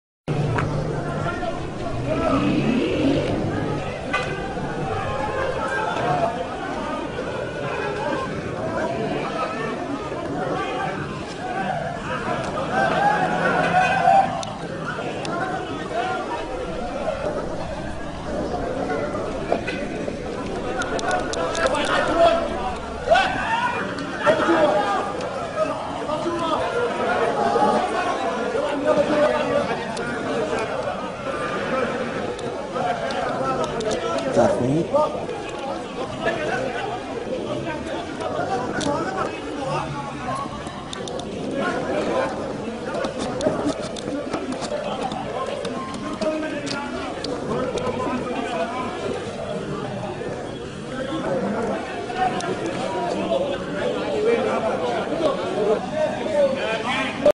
Al-Assad's gangs beat a protester and took him to a torture cellar, Aleppo, Syria 11.11.2011
Al-Assad's gangs beat a protester and took him to a torture cellar in Aleppo city in Syria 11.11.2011